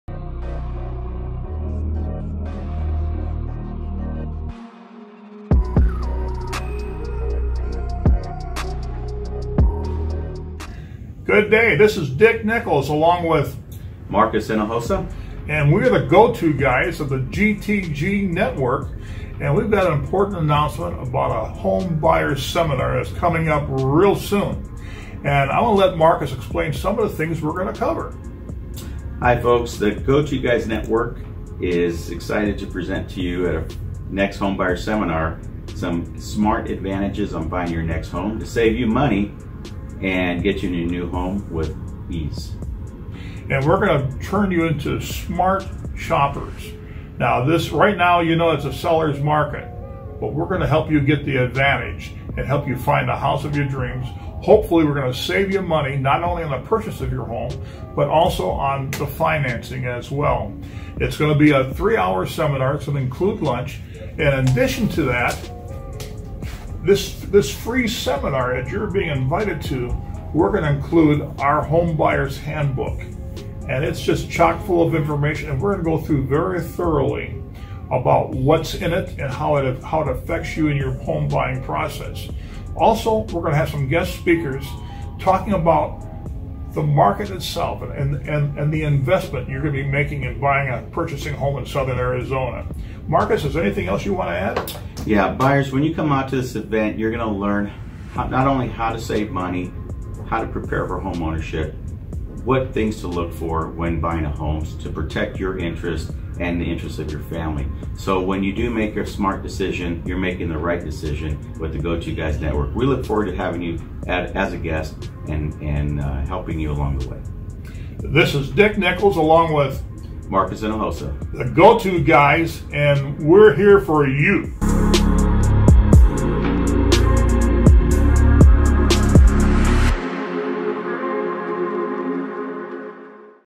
The Number 1 Home Buyer's Seminar In Tucson...and it's FREE!
Learn how to be "Smart Buyers" when it comes to buying & financing a home in Southern Arizona
Presented By: Dick Nichols Realtor & Marcus Hinojosa Lending Expert